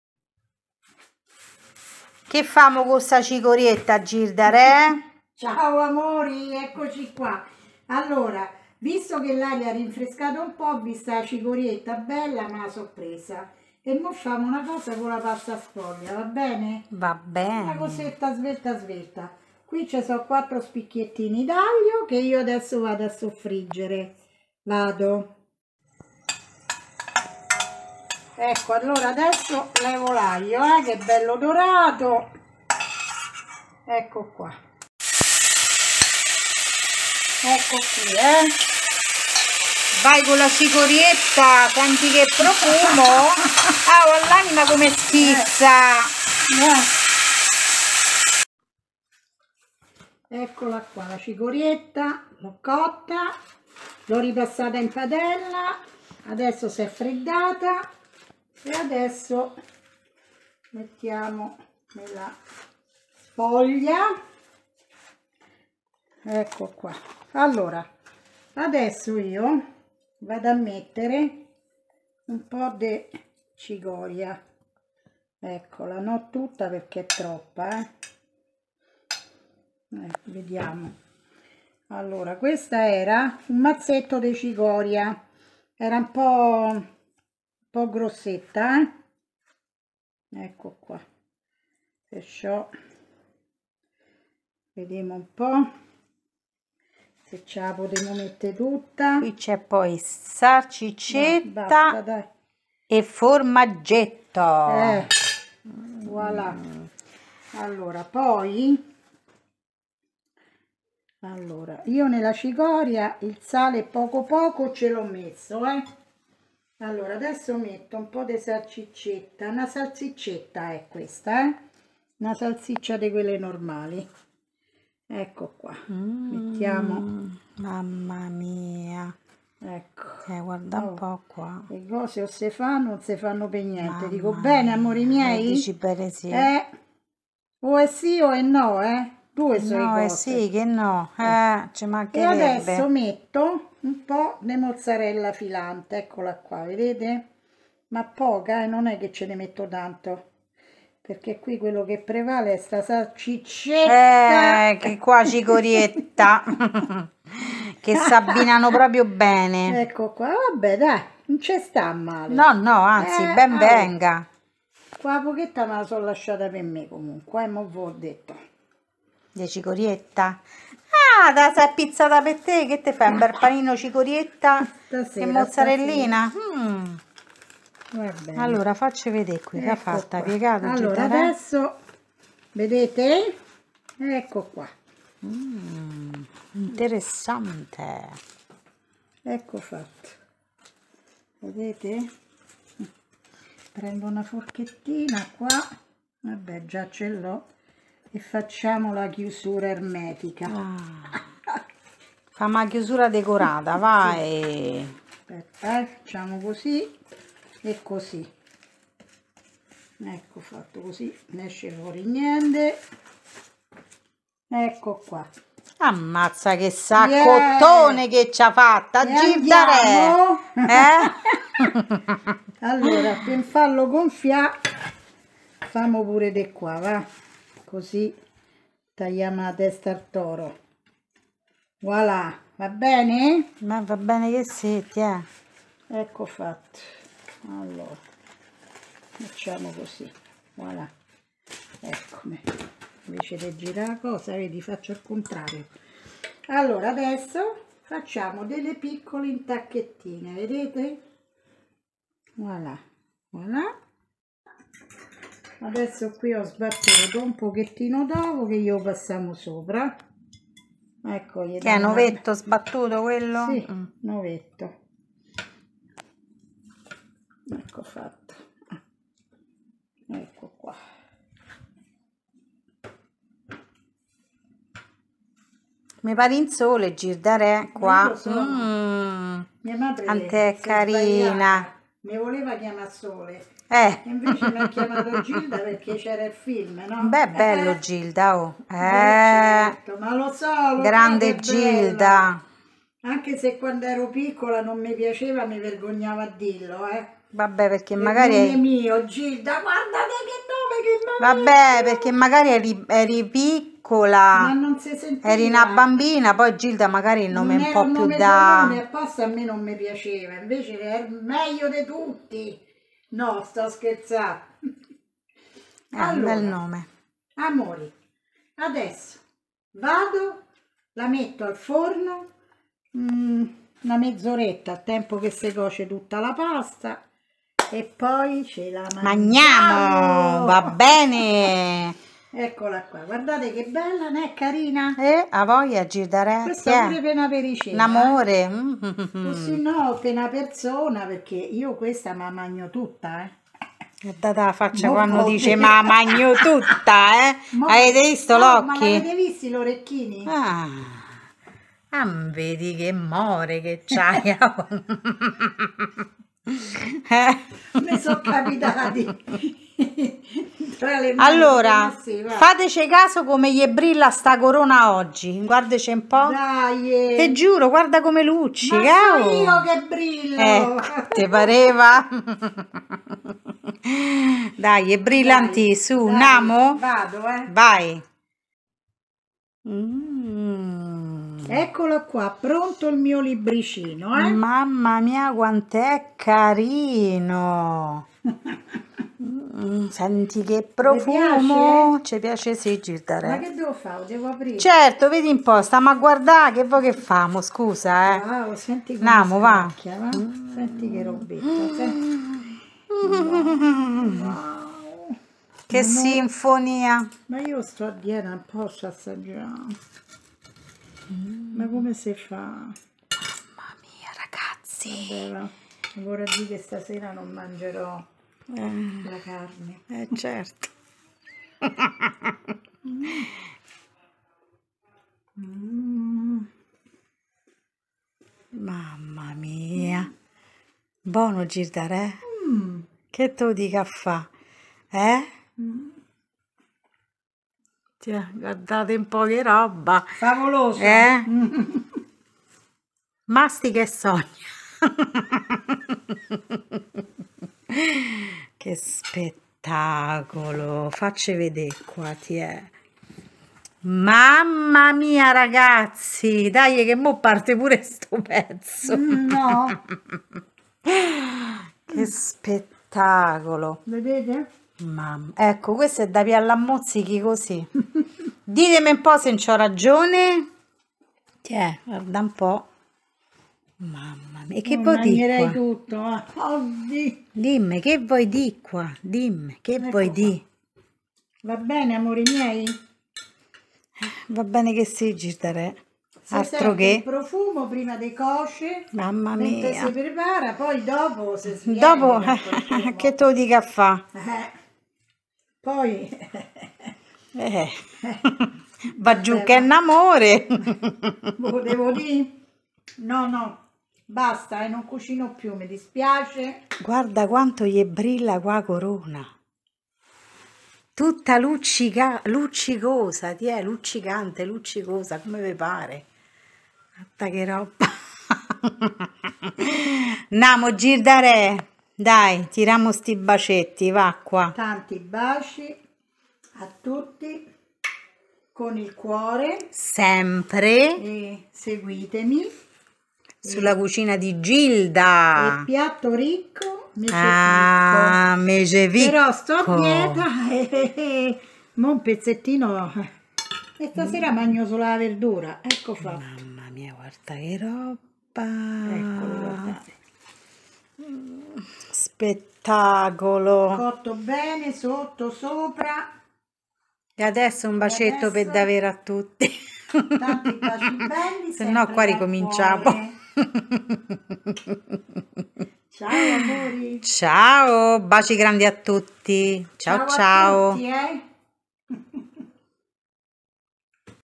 QUADROTTO RUSTICO RIPIENO DI CICORIA E SALSICCIA TROPPO BUONO A TUTTE LE ORE
Quadrotto rustico ripieno di cicoria e salsiccia.
Molto sfizioso e semplicissimo da fare, questo ci piace davvero tanto tanto😁

📌Ingredienti:
1 rotolo di pasta sfoglia rettagonlare
4 spicchi di aglio
olio evo q.b.
sale q.b.
1 mazzetto di cicoria
1 salsiccia
qualche pezzetto di mozzarella a pasta filata
Per il procedimento seguite il video❤